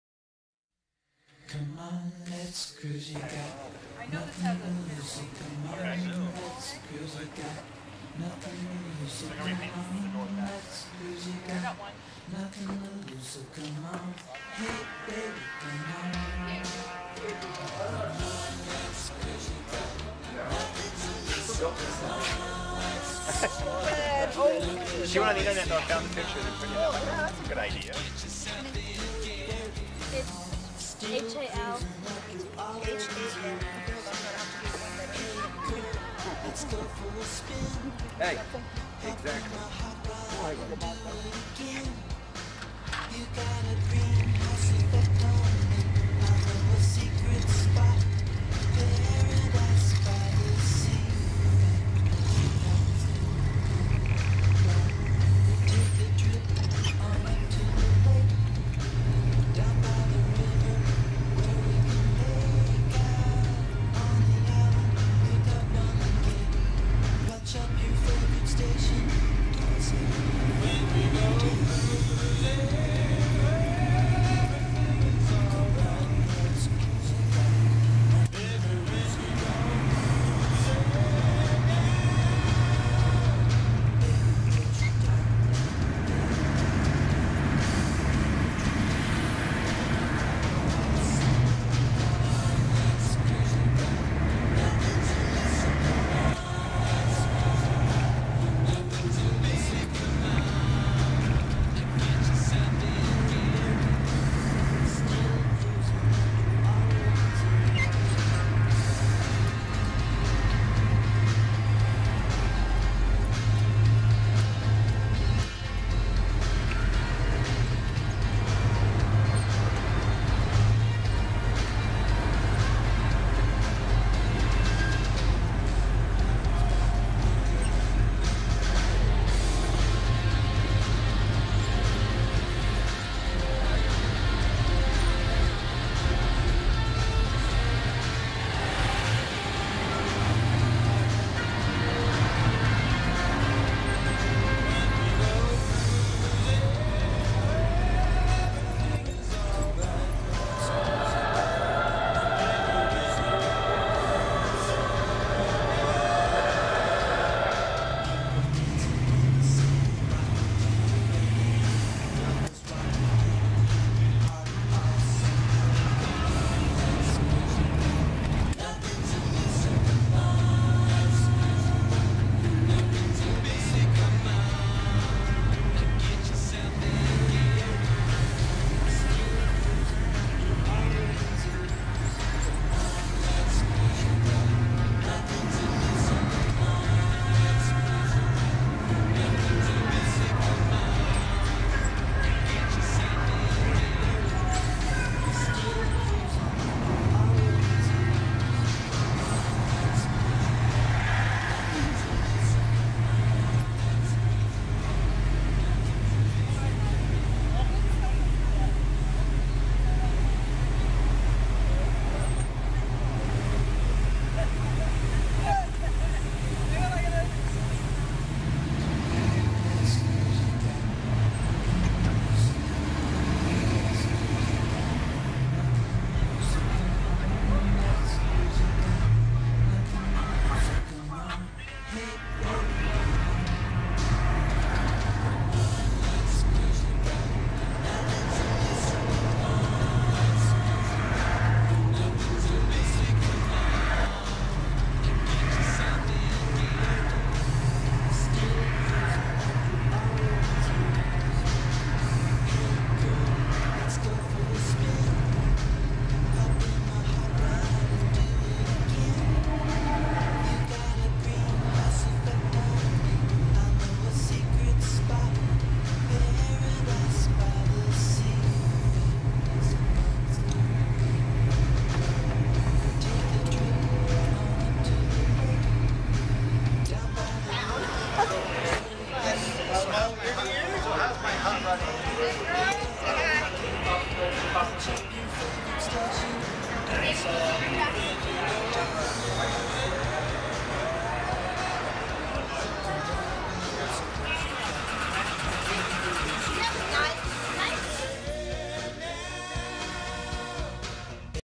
Cruzzin the Strip with the Deuce of Spades
Cruzzin the Vegas Strip with Faith Granger of the Deuce of Spades and the boyz from Old School Hot Rods